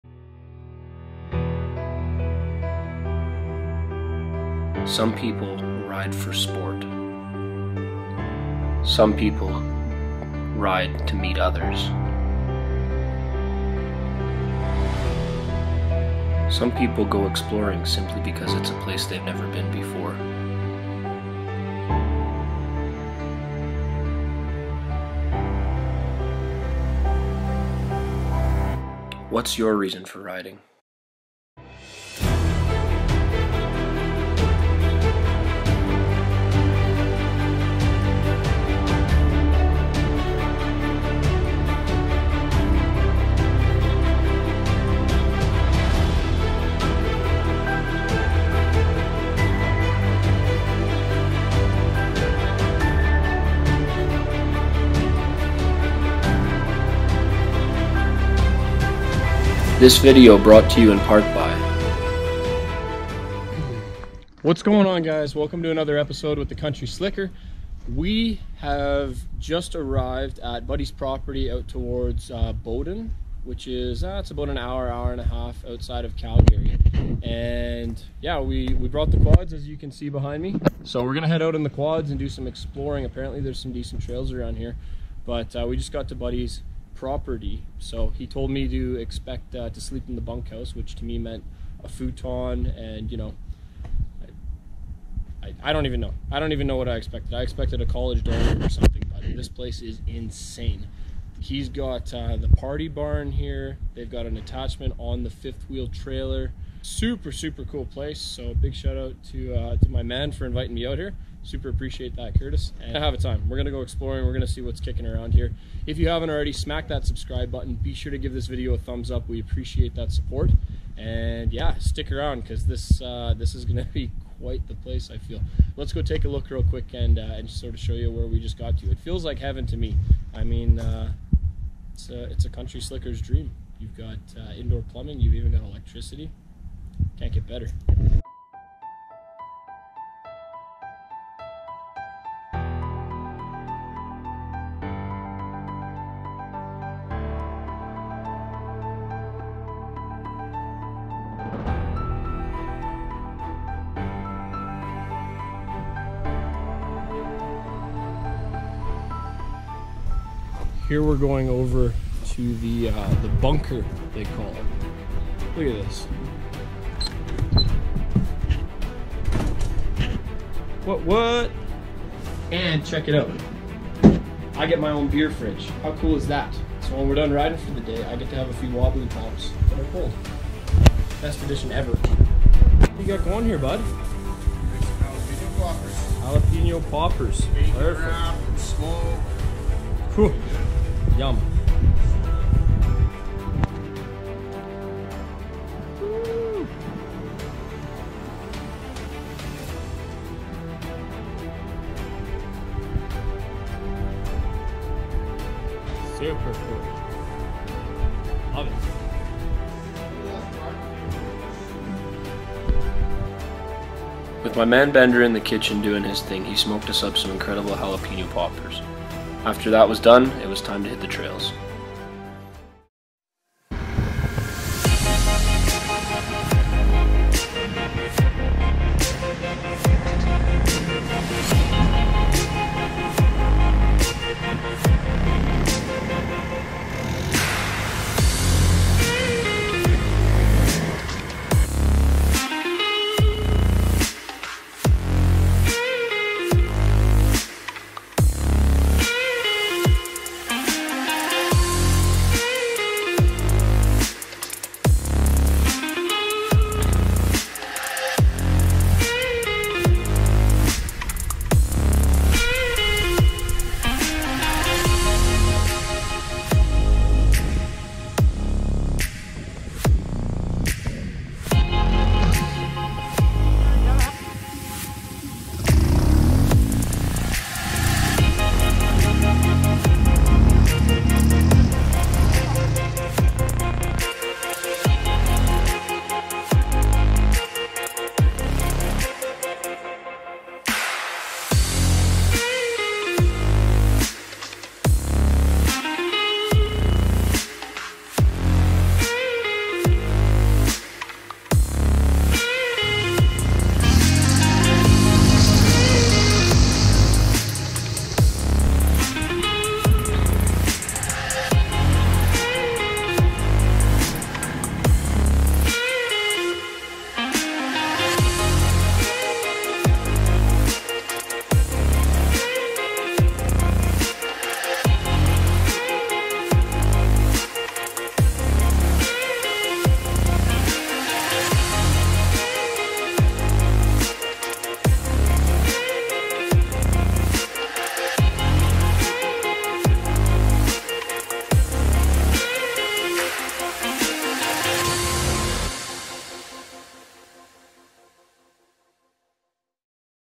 Red Deer River | Epic ATV offroad trip
We explore into a new area discovering the atv trails on the red deer river. This extensive trail system is pure heaven for an offroad trip featuring mostly rocky/ sandy trails that weave along the red deer river and duck in and out of the adjacent bush. Nice to know we have yet another cool area to ride close to home.